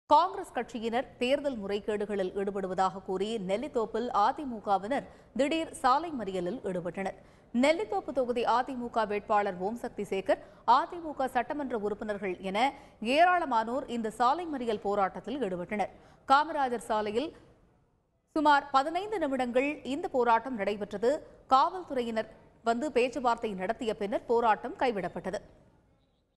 "Congress violates election rules" - AIADMK accuses & stage road blockade in Nellithope
"Congress violates election rules" - AIADMK accuses & stage road blockade in Nellithope constituency